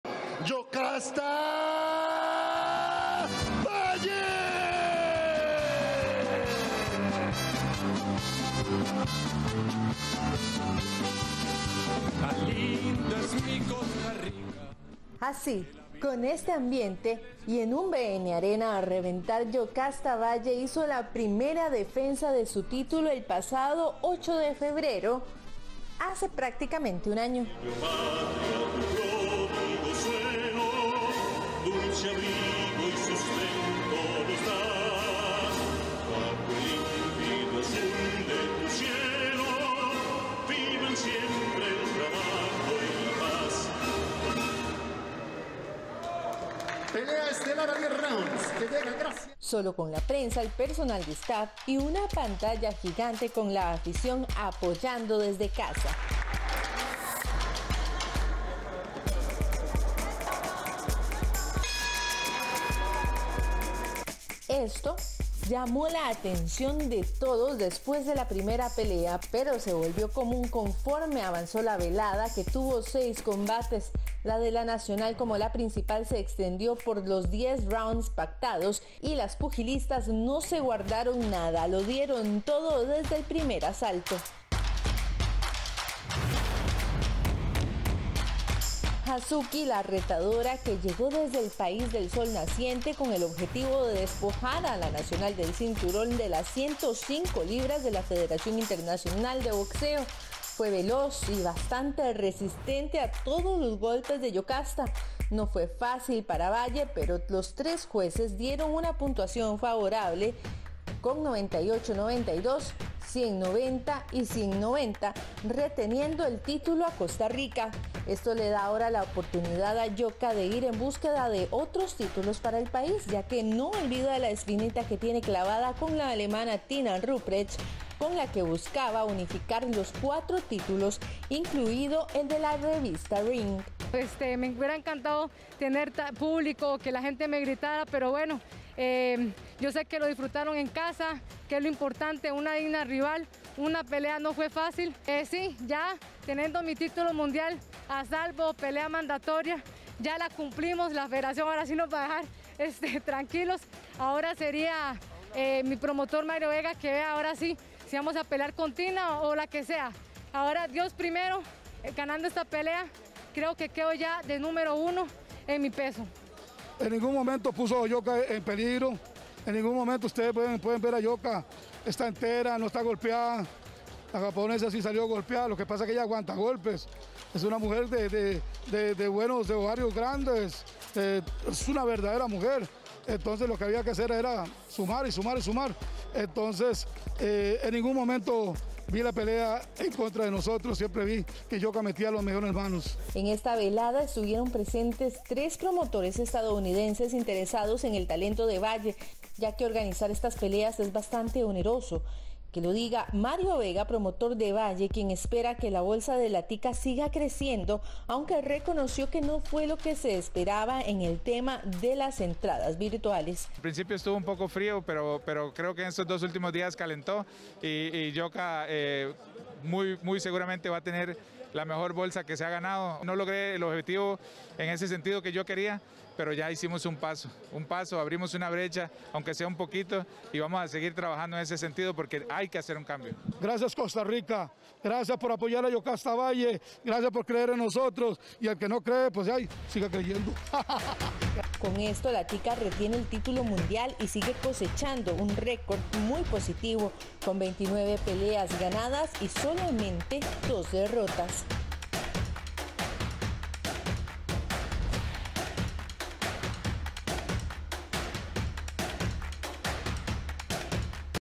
Yokasta Valle retuvo su título en las 105 libras de la federación mundial de
YOKASTA VALLE RETUVO SU TÍTULO EN LAS 105 LIBRAS DE LA FEDERACIÓN MUNDIAL DE BOXEO EN LA MANDATORIA ANTE SANA HAZUKI LA NOCHE DEL SABADO, EN UNA PELEA EN LA QUE LA PÚGIL ENVÍO UN MENSAJE PARA LUCHAR CONTRA EL FEMINICIDIO Y TAMBIÉN LA BRECHA DE GÉNERO EN EL DEPORTE.